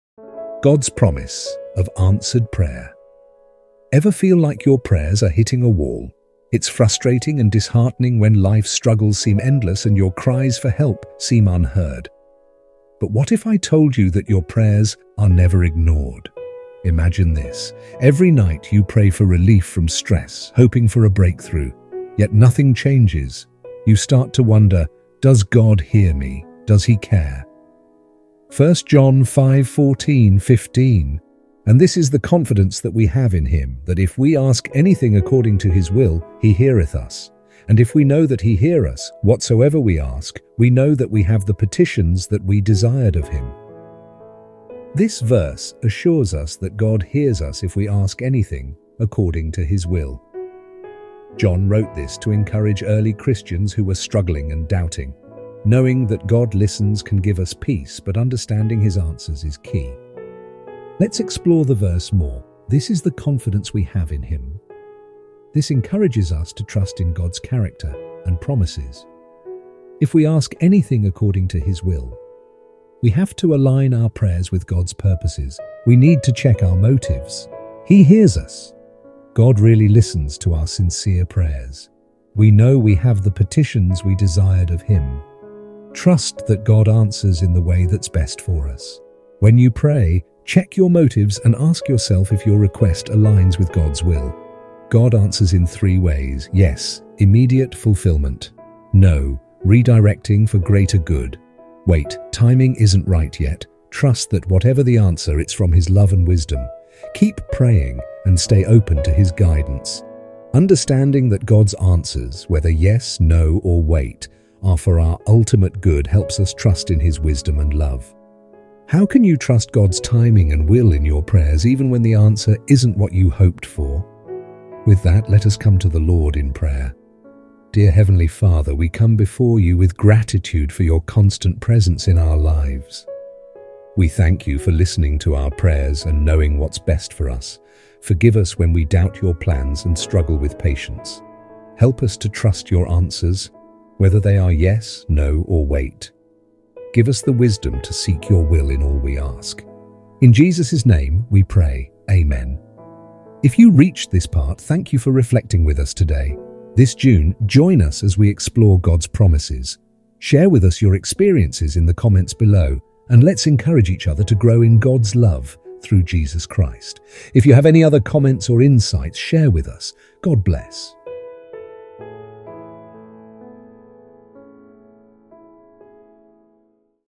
Why Aren’t My Prayers Answered? | BDS Daily Devotional | God’s Promises
Devotional of the day:

Ever feel like your prayers are hitting a wall? It’s frustrating when life’s struggles seem endless and our cries for help seem unheard. Imagine praying every night for relief, yet nothing changes. We start to wonder, “Does God hear us? Does He care?”

In this video, we explore the assurance that God hears us when we pray. We’ll uncover the secrets of aligning our prayers with God’s purpose and understanding His responses—whether Yes, No, or Wait. Discover how these insights can bring us peace and trust in God’s wisdom.

Join us to learn practical steps and deep reflections that can transform your prayer life. Watch till the end for a heartfelt prayer and encouraging message.

Key Takeaways:

 • Confidence in God’s listening.
 • Aligning our prayers with His will.
 • Trusting God’s timing and answers.

Don’t miss this chance to deepen your faith and find comfort in knowing God hears you.